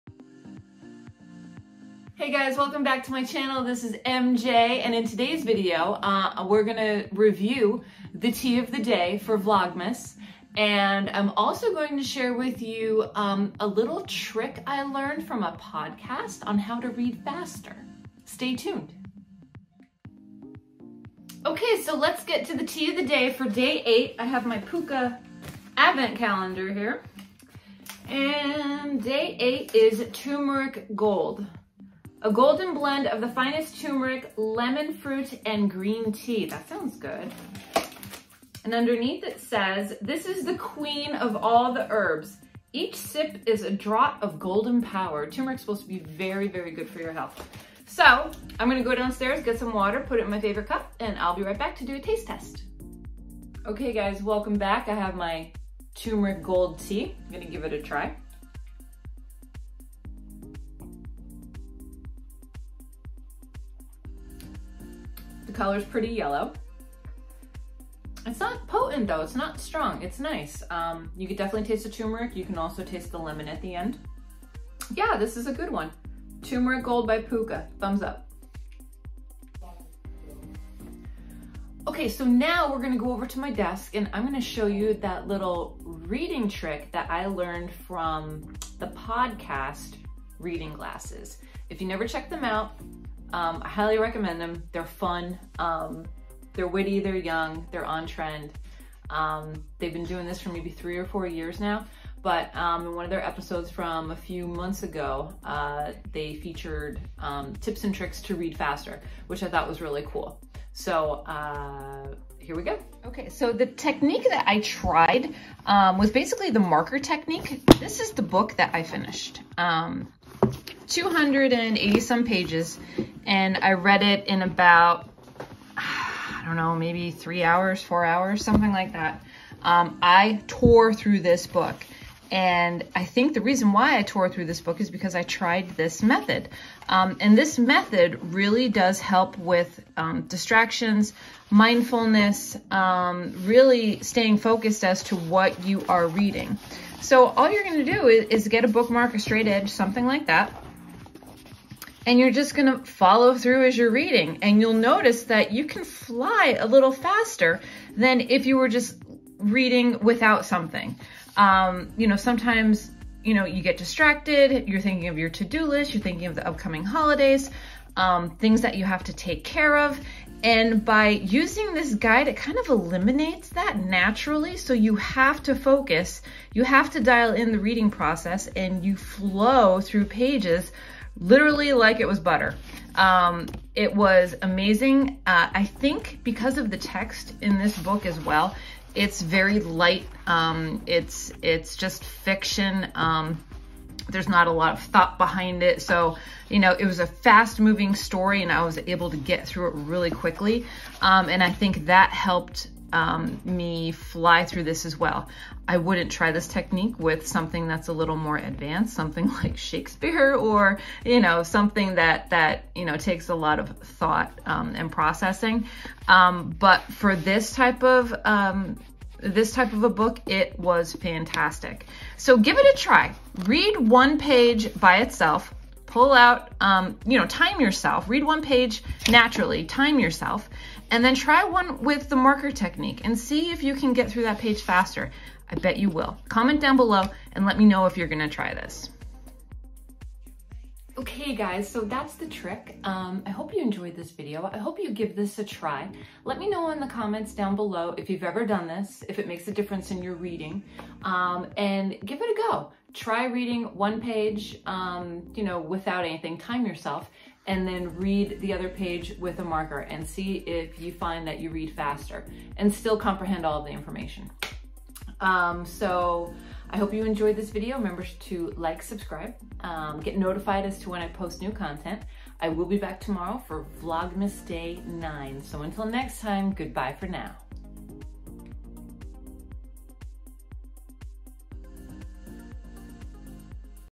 Tea + Read Faster Hack
Join me for Vlogmas Day 8 with the tea of the day and a hack to increase your reading speed.  Thanks for stopping by!